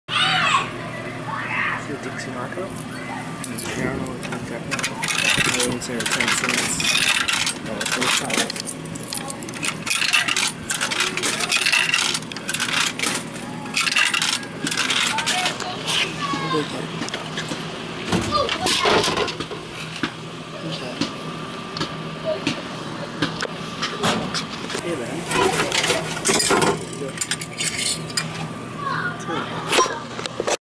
Dixie Narco E Series Vending Machine @ Some Fred Bus Stop, Fredericksburg, VA| TheLilRubix1 Videos
$1.25 isn't bad, cheaper than Wall Mart, but the soda tasted a little funny...